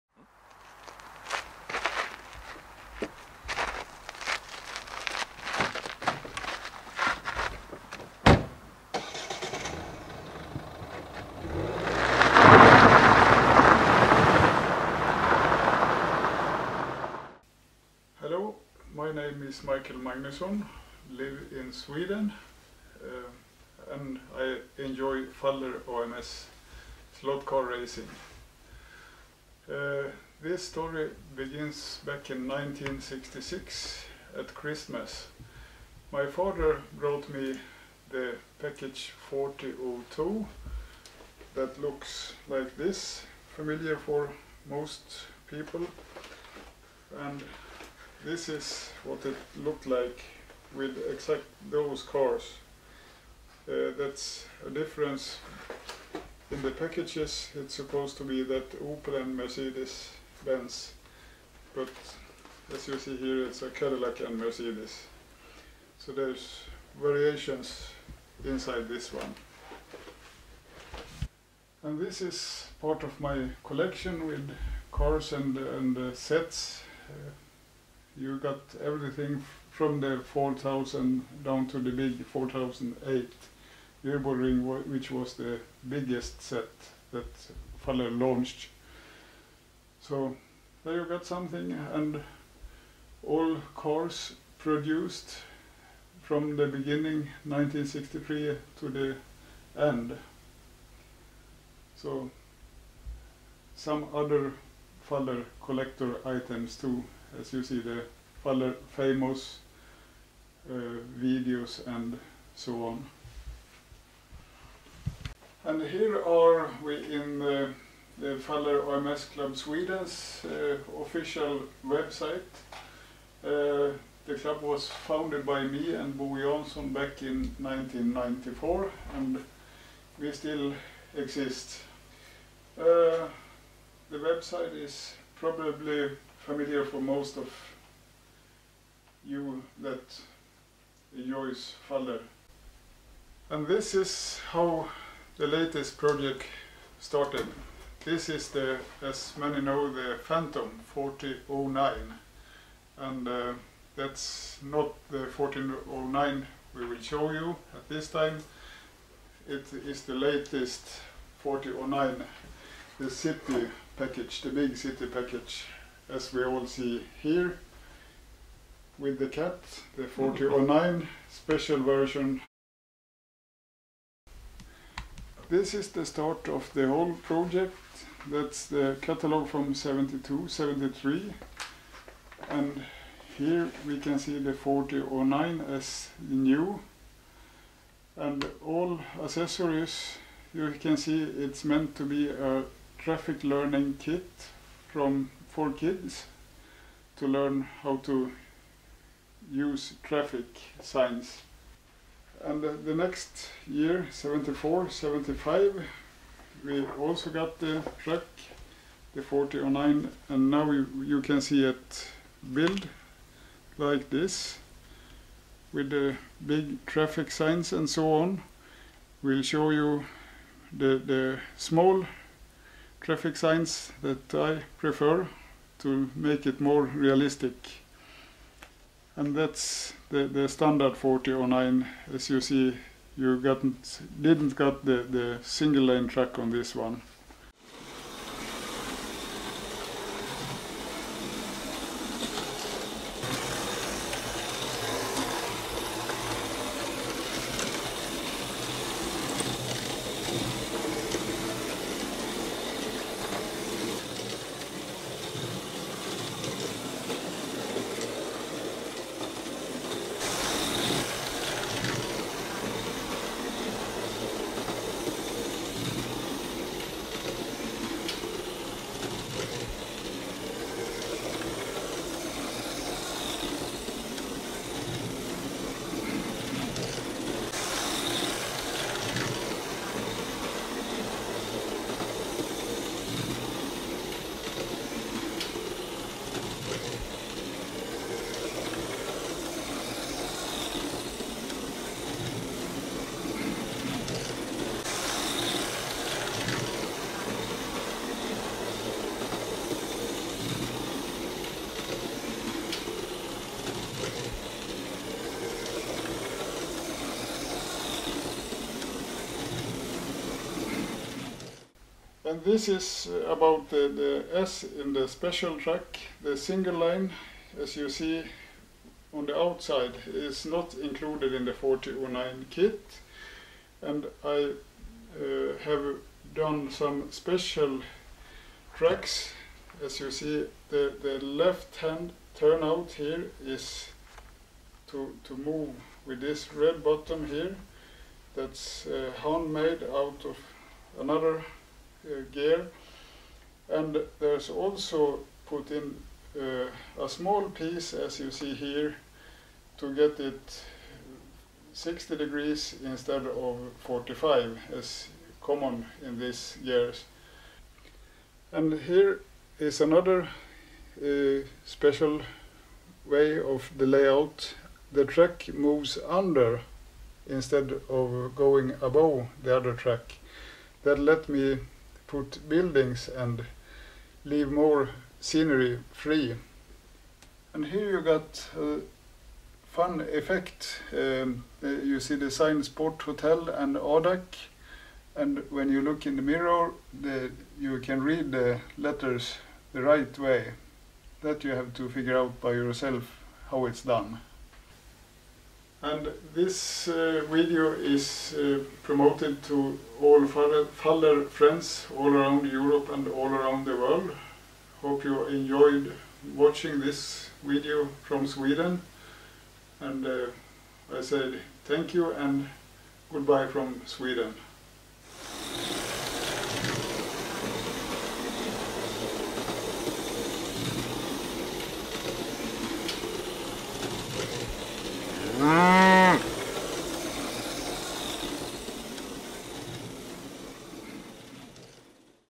MJ-inspektionen visits a Faller AMS-layout in Sweden.
Michael Magnusson tells us about his life with Faller slot cars.